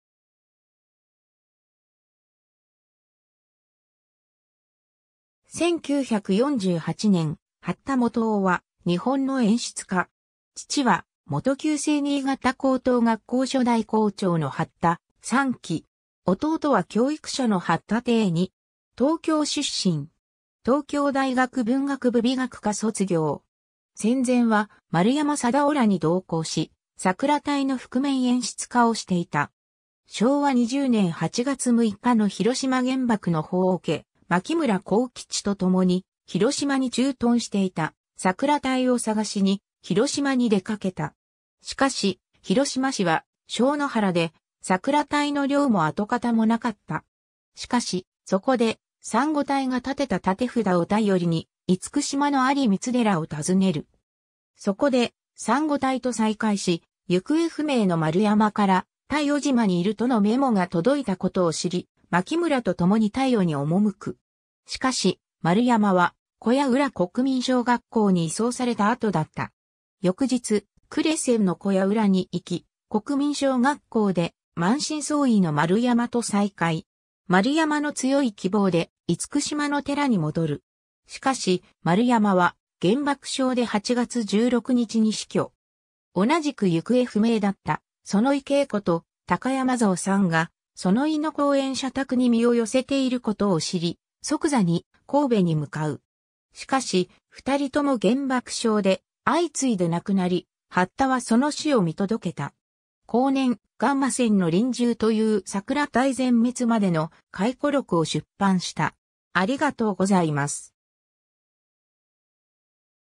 八田元夫
1948年 八田 元夫（はった もとお、1903年11月13日　- 1976年9月17日）は、日本の演出家。
父は元旧制新潟高等学校初代校長の八田三喜。
弟は教育者の八田亭二。
 東京出身。
東京大学文学部美学科卒業。
 戦前は丸山定夫らに同行し、桜隊の覆面演出家をしていた。
昭和20年8月6日の広島原爆の報を受け、槙村浩吉と共に広島に駐屯していた桜隊を探しに広島に出かけた。
 しかし、広島市は焼野原で桜隊の寮も跡形もなかった。
しかし、そこで珊瑚隊が立てた立札を頼りに厳島の存光寺を訪ねる。
そこで珊瑚隊と再会し、行方不明の丸山から「鯛尾島にいる」とのメモが届いたことを知り、槙村とともに鯛尾に赴く。
しかし、丸山は小屋浦国民小学校に移送された後だった。
翌日、呉線の小屋浦に行き、国民小学校で満身創痍の丸山と再会。
丸山の強い希望で、厳島の寺に戻る。
しかし、丸山は原爆症で8月16日に死去。
同じく行方不明だった園井恵子と高山象三が、園井の後援者宅に身を寄せていることを知り、即座に神戸に向かう。
しかし、2人とも原爆症で相次いで亡くなり、八田はその死を見届けた。
後年、「ガンマ線の臨終」という桜隊全滅までの回顧録を出版した。